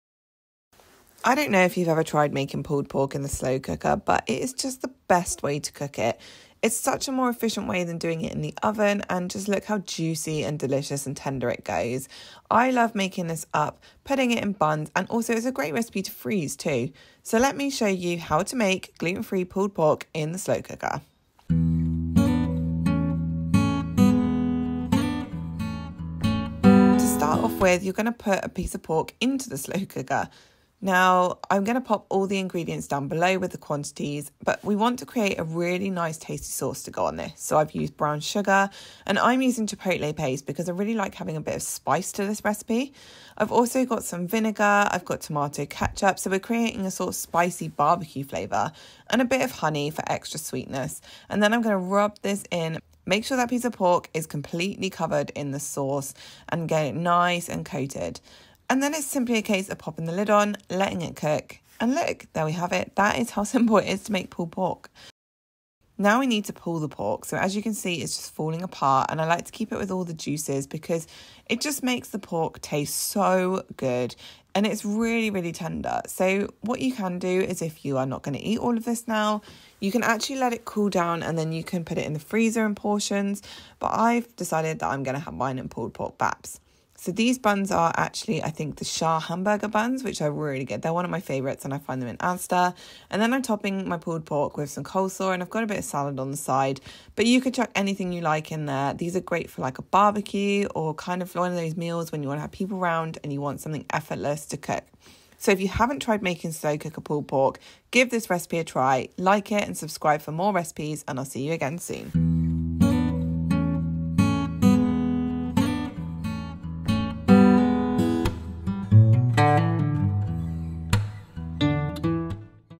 Slow Cooker Pulled Pork Recipe (Gluten Free)
I love gluten free pulled pork but making it in the slow cooker is a much easier and more affordable way to do it. Slow cooker pulled pork is so tasty, especially with the addition of chipotle for extra flavour!

INGREDIENTS

1.3kg pork shoulder
75g chipotle paste
2 tbsp honey
2 tbsp dark brown sugar
2 tbsp tomato ketchup
1 tbsp apple cider vinegar

New vlogs uploaded every Saturday at 8am - when I remember!! x